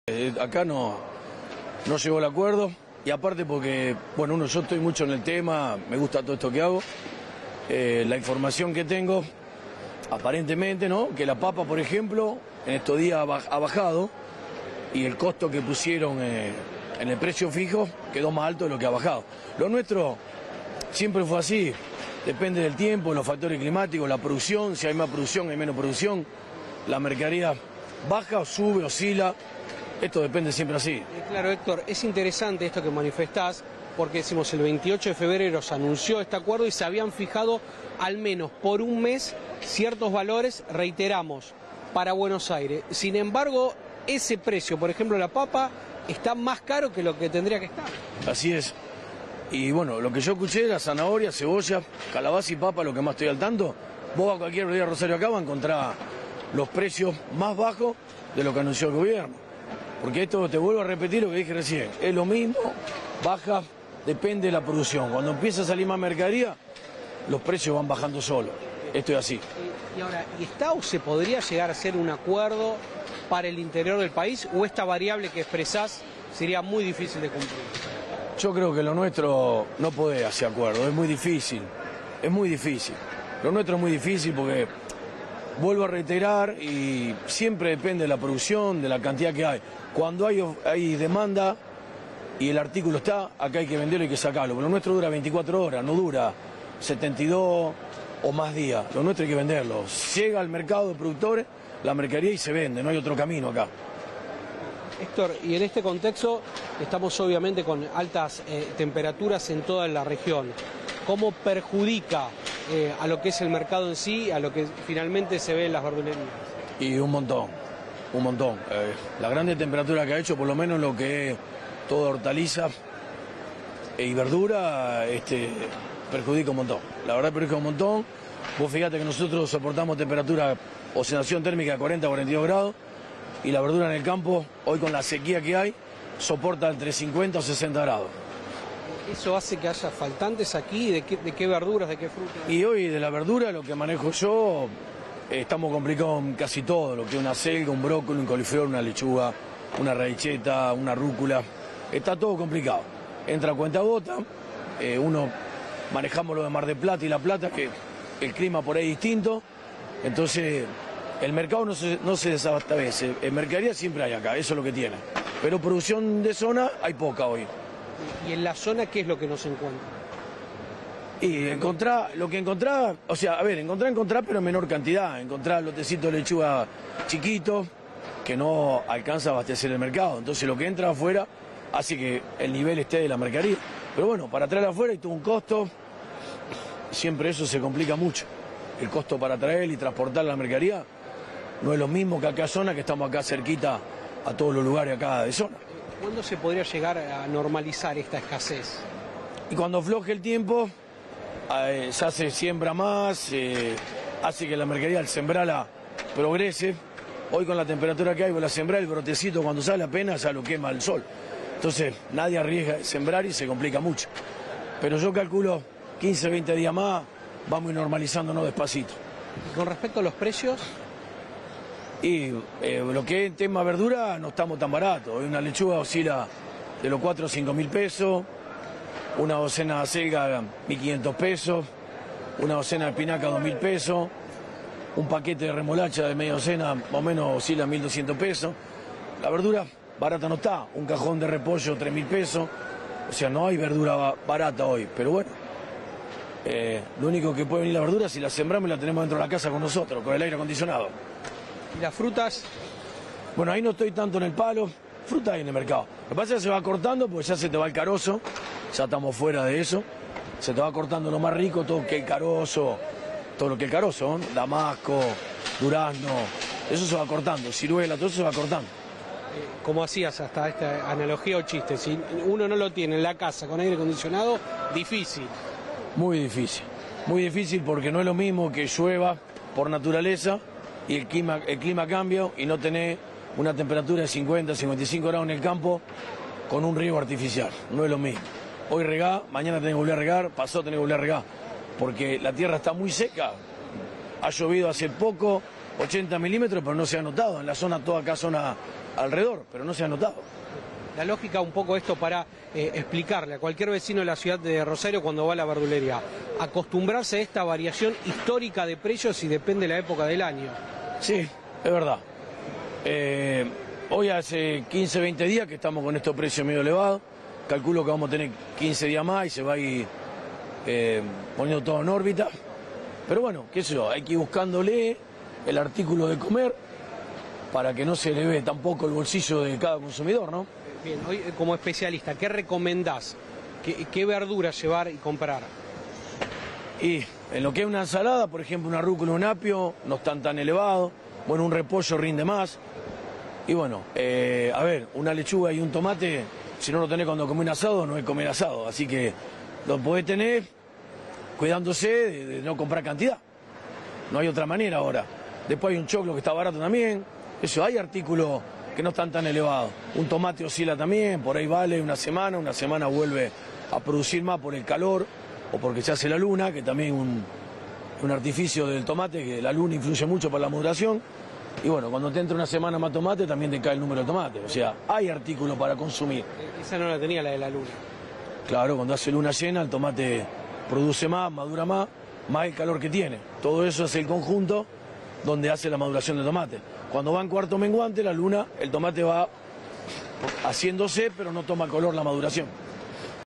Congelamiento de precios: el plan del Gobierno para frutas y verduras no llegó a la ciudad
💬 "En estos días, el precio de la papa bajó y el que pusieron como precio fijo quedó más alto", indicó Héctor Mariani, del Mercado de Productores.